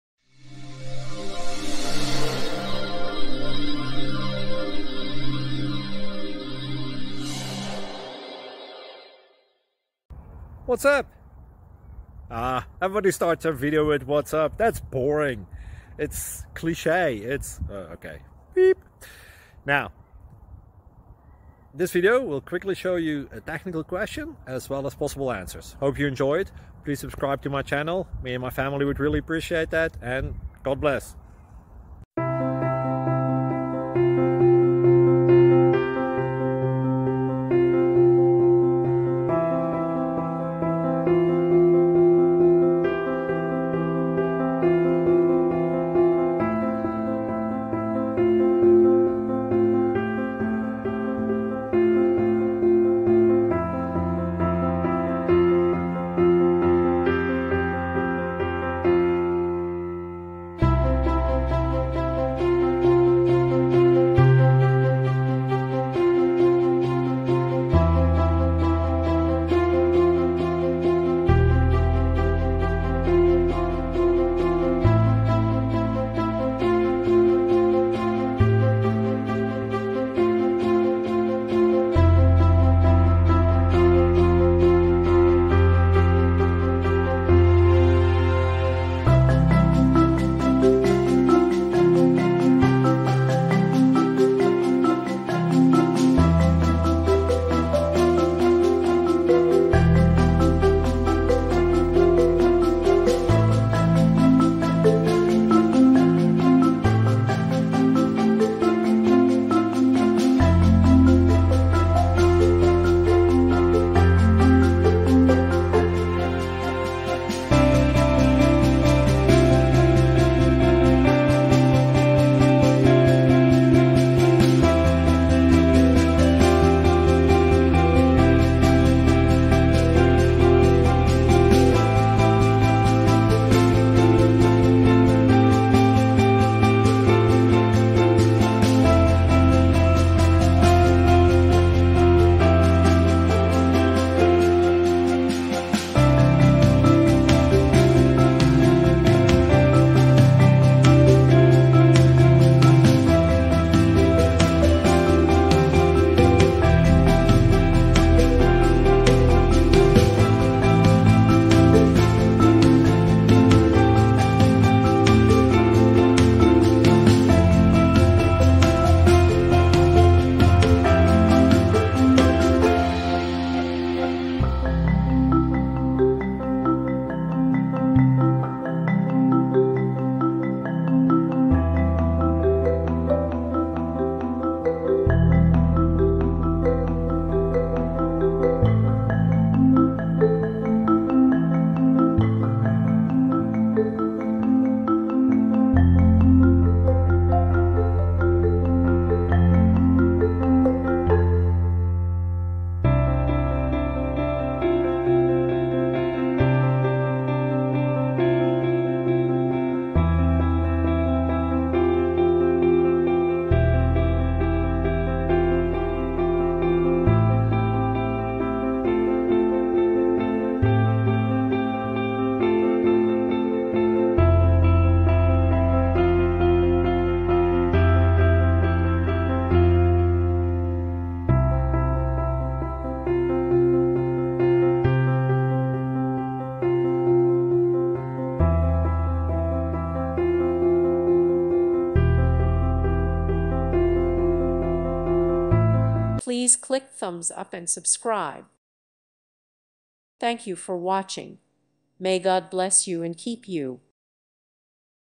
Code Review: A GameGrid class to hold the underlying data for grid-based game (2 Solutions!!)
Code Review: A GameGrid class to hold the underlying data for grid-based game